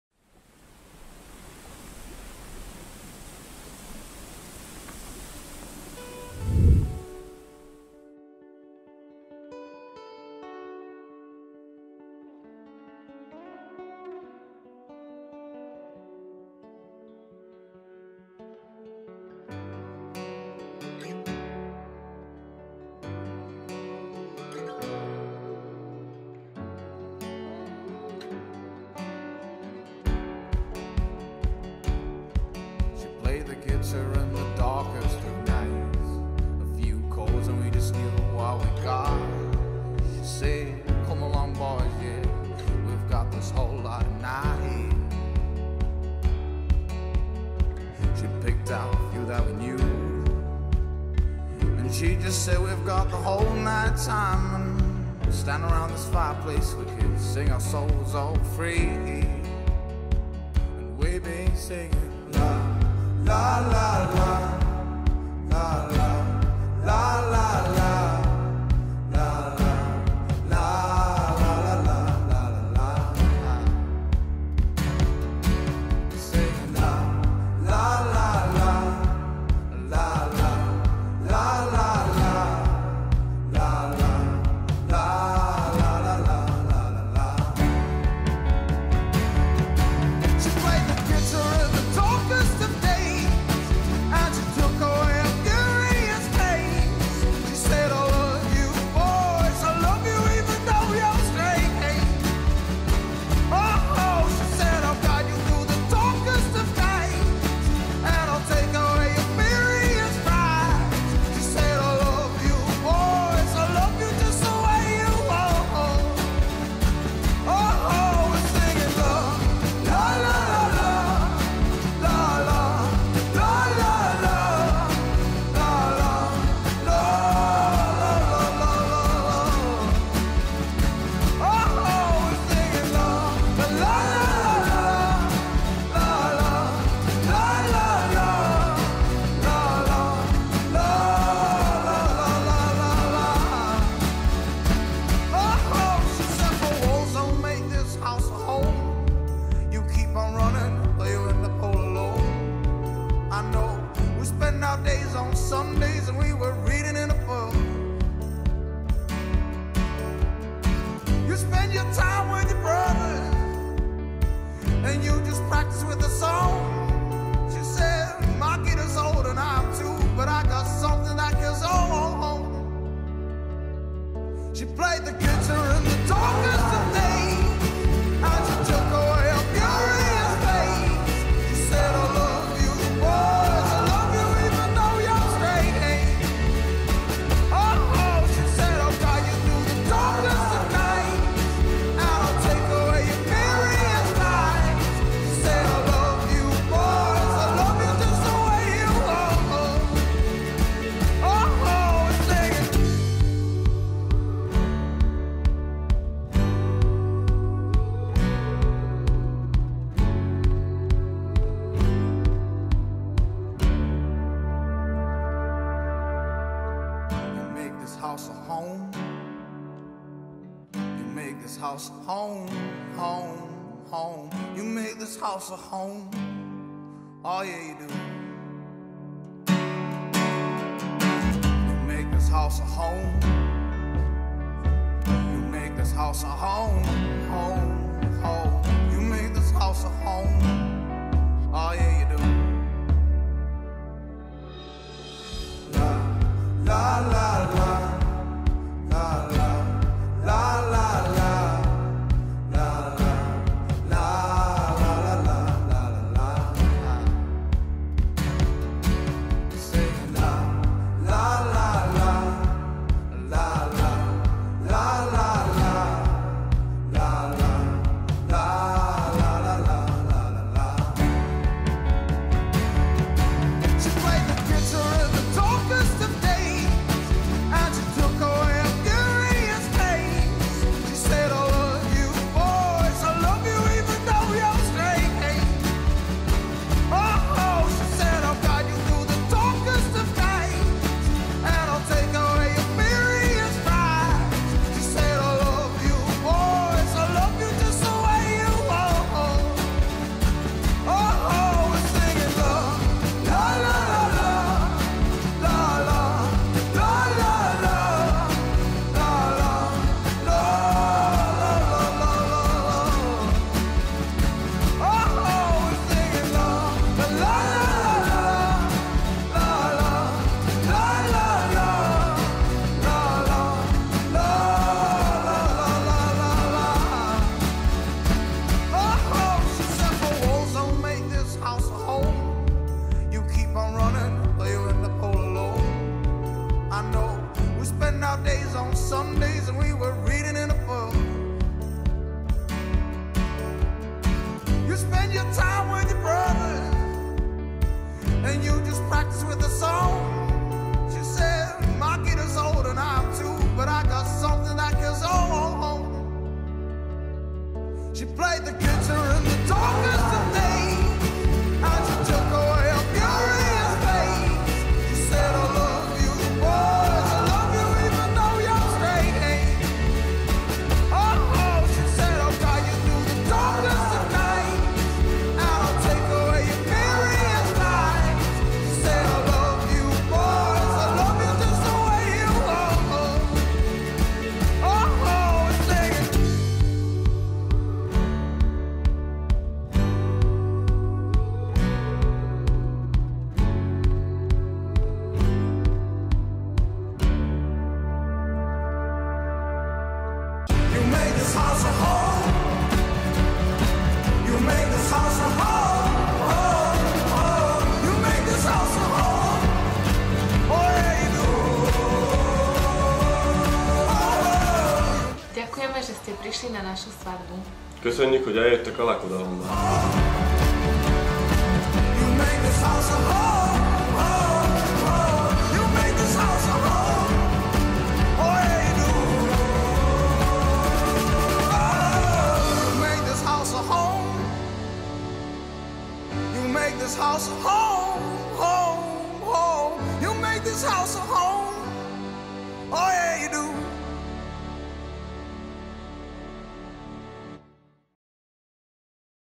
Veronika a Lacko - Wedding highlights
Aj keď nám počas dňa zahrmelo, táto svadba dopadla nadmieru dobre. Priam vynikajúco. Veď keď sa nevesta so ženíchom smejú a je skvelá zábava, tak aj robota ide lepšie. Veronika a Lacko prajeme vám len to najlepšie a ten úsmev nech vám neopadne aj keď raz za čas príde nejaký ten oblak.